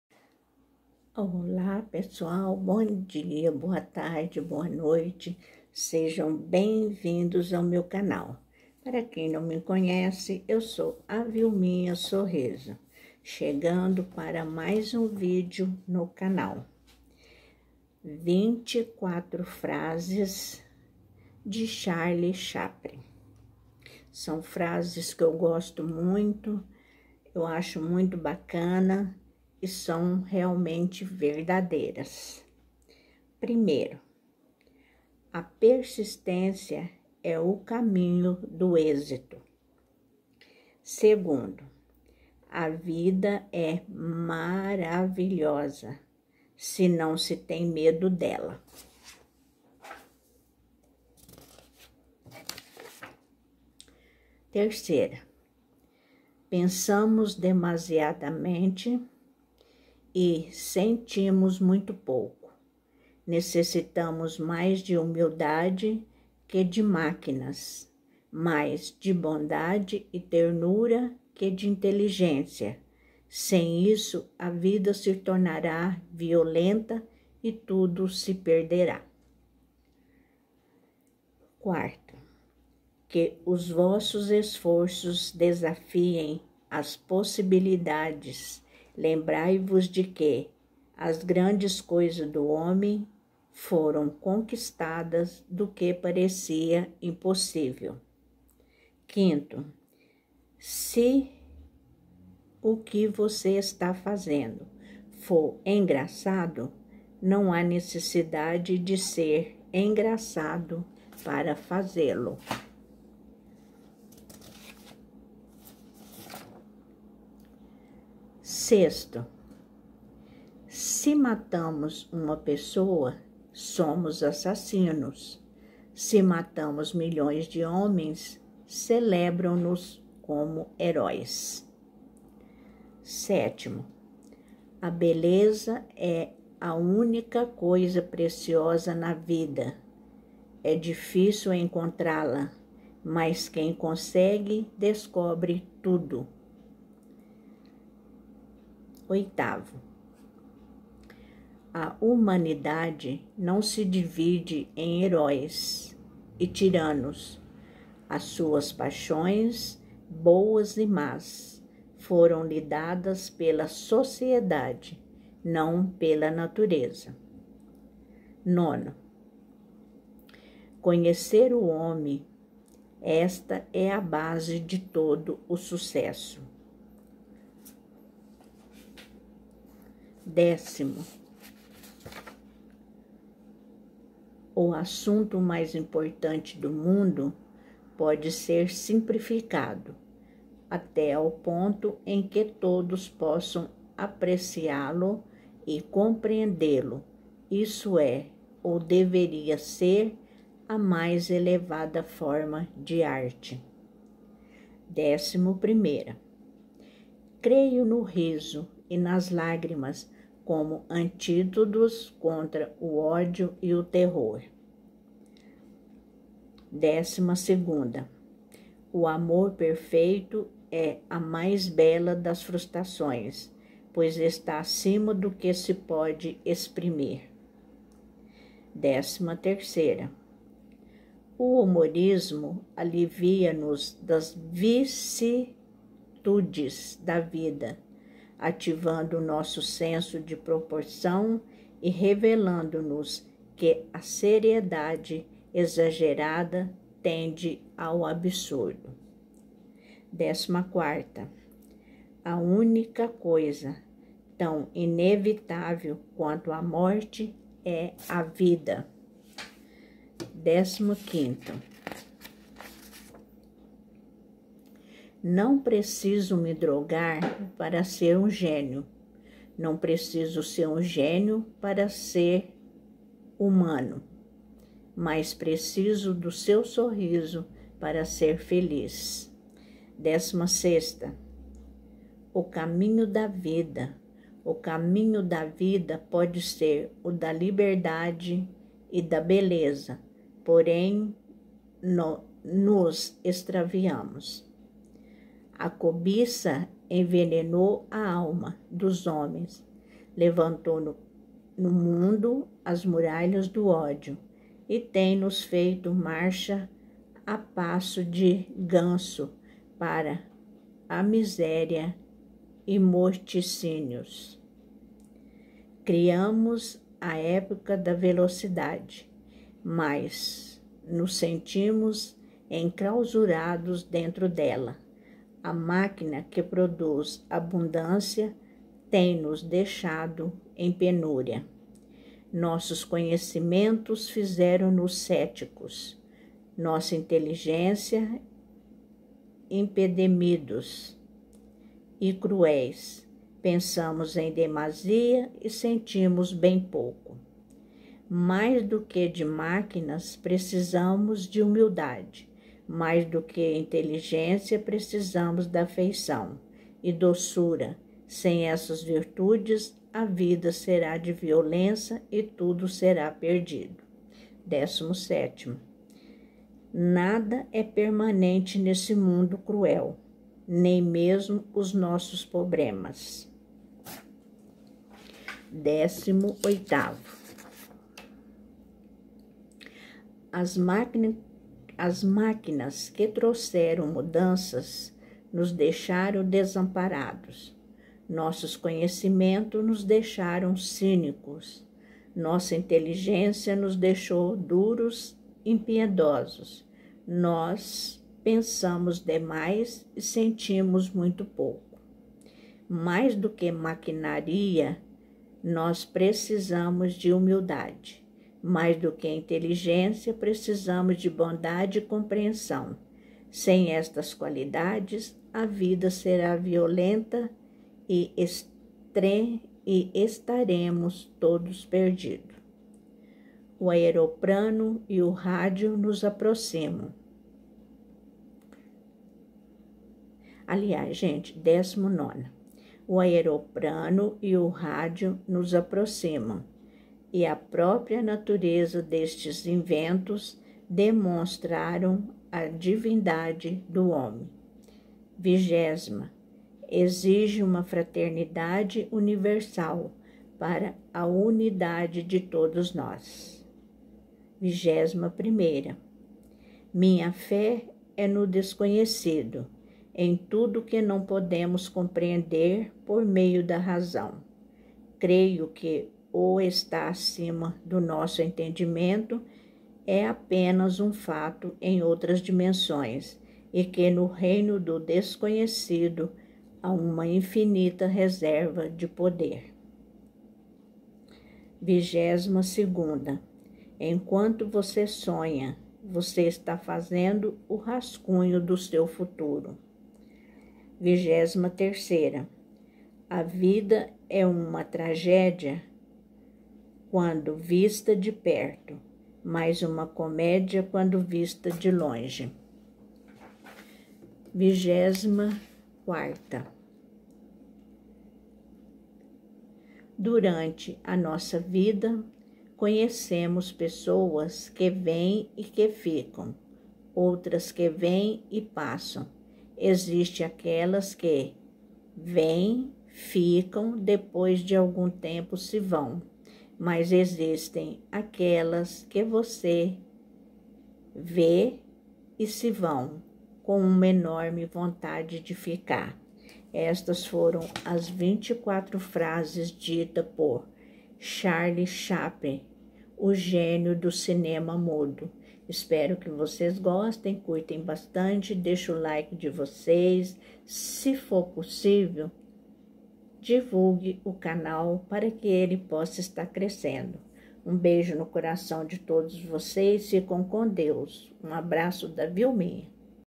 # Vinte e quatro frases ditas por CHARLIE SHAPLIM...
Vinte e quatro frases ditas pelo Artista do Cinema Mudo, nosso querido e saudoso CHARLIE CHAPLIM...são frases lindas e verdadeiras...Eu curto e recomendo que: ouçam com cuidado é analise frase a frase e vejam como elas são verdadeiras pérolas pra nossa vida...